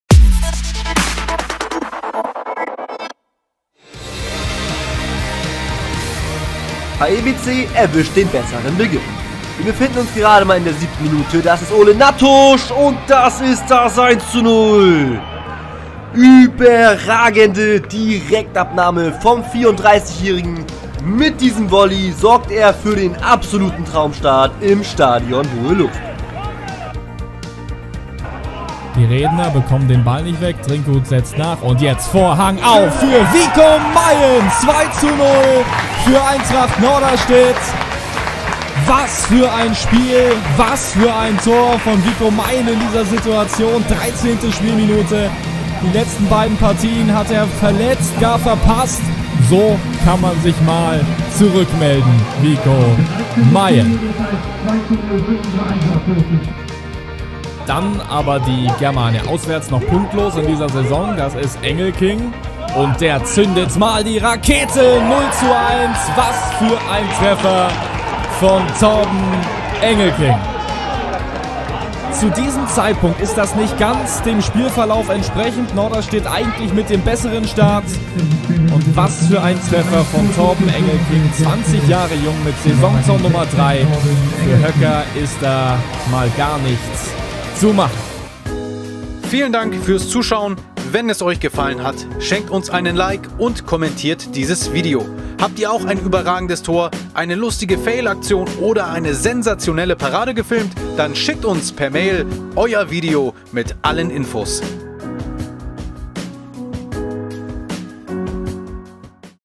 Sturm an der Elbe! | TOP-Tore November 2018
Die Stürmer im Norden machen's volley und aus der Distanz!

Platz 3 ► Den trifft er perfekt! Traumvolley von Ole Natusch (HEBC)

Platz 2 ► Volleyabnahme | Geiles Tor von Vico Meien (Eintracht Norderstedt)

Platz 1 ► Was für eine Rakete | Tor von Torben Engelking (1. FC Germania Egestorf/Langreder)